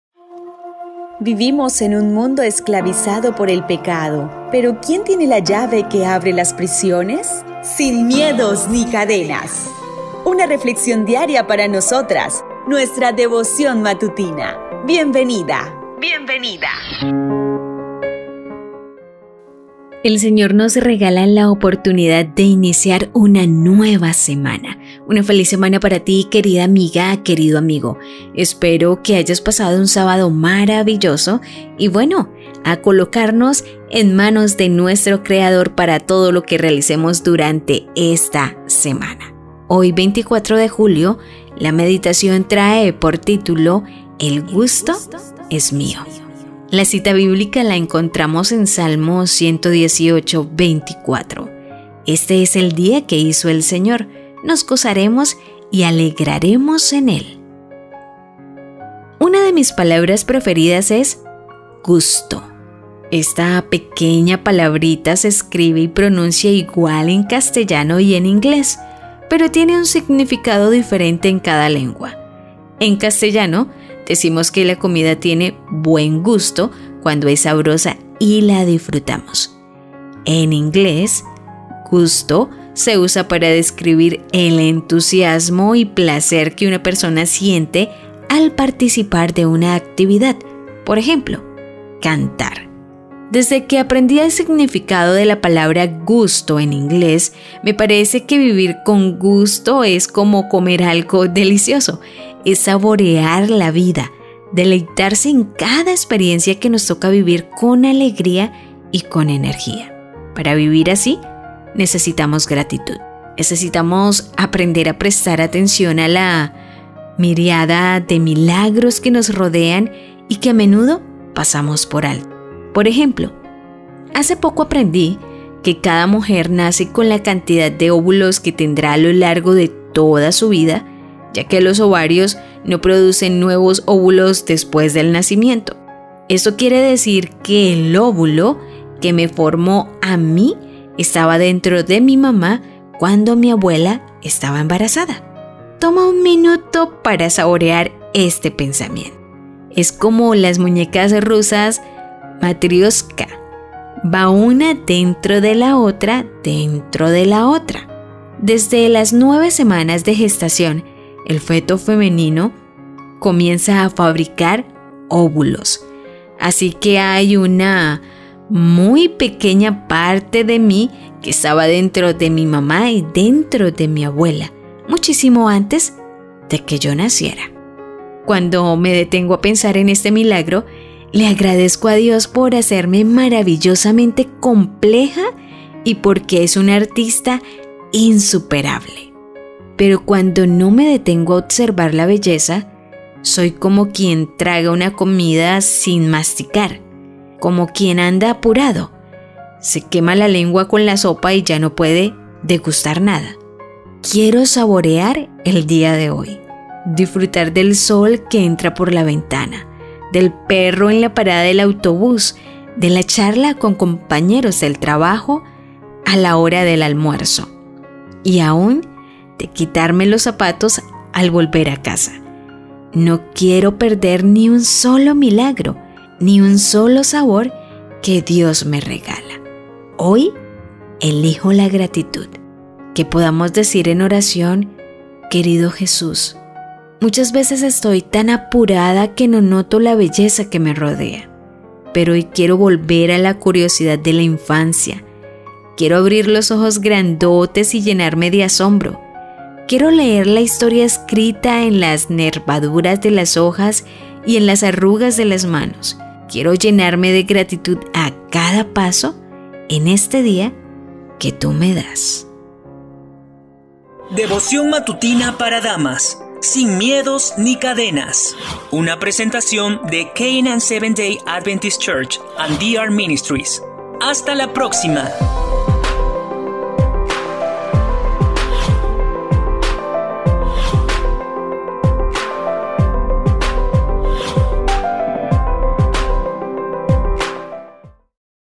Devoción Matutina Para Damas - 24 de Julio del 2022
24 DE JULIO
EL GUSTO ES MÍO
"Este es el día que hizo el Señor, nos gozaremos y alegraremos en él” (Sal. 118:24).
Una de mis palabras preferidas es “gusto”. Esta pequeña palabrita se escribe y pronuncia igual en castellano y en inglés, pero tiene un significado diferente en cada lengua. En castellano, decimos que la comida tiene buen gusto cuando es sabrosa y la disfrutamos. En inglés, gusto se usa para describir el entusiasmo y placer que una persona siente al participar de una actividad, por ejemplo: cantar.
Desde que aprendí el significado de la palabra gusto en inglés, me parece que vivir con gusto es como comer algo delicioso. Es saborear la vida, deleitarse en cada experiencia que nos toca vivir con alegría y con energía. Para vivir así, necesitamos gratitud. Necesitamos aprender a prestar atención a la miríada de milagros que nos rodean y que a menudo pasamos por alto.
Por ejemplo, hace poco aprendí que cada mujer nace con la cantidad de óvulos que tendrá a lo largo de toda su vida, ya que los ovarios no producen nuevos óvulos después del nacimiento. Esto quiere decir que ¡el óvulo que me formó a mí estaba dentro de mi mamá cuando mi abuela estaba embarazada! Toma un minuto para saborear este pensamiento. Es como las muñecas rusas matrioshka: va una dentro de la otra, dentro de la otra... Desde las nueve semanas de gestación, el feto femenino comienza a “fabricar” óvulos. Así que hay una muy pequeña parte de mí que estaba dentro de mi mamá y dentro de mi abuela, muchísimo antes de que yo naciera.
Cuando me detengo a pensar en este milagro, le agradezco a Dios por hacerme maravillosamente compleja, y porque es un Artista insuperable (Sal. 139:14). Pero cuando no me detengo a observar la belleza, soy como quien traga una comida sin masticar; como quien anda apurado, se quema la lengua con la sopa y ya no puede degustar nada.
Quiero saborear el día de hoy, disfrutar del sol que entra por la ventana, del perro en la parada del autobús, de la charla con compañeros del trabajo a la hora del almuerzo, y aun de quitarme los zapatos al volver a casa. No me quiero perder ni un solo milagro, ni un solo sabor que Dios me regala. Hoy elijo la gratitud.
Querido Jesús, muchas veces estoy tan apurada que no noto la belleza que me rodea. Pero hoy quiero volver a la curiosidad de la infancia. Quiero abrir los ojos grandotes y llenarme de asombro. Quiero leer la historia escrita en las nervaduras de las hojas y en las arrugas de las manos. Quiero llenarme de gratitud a cada paso en este día que tú me das.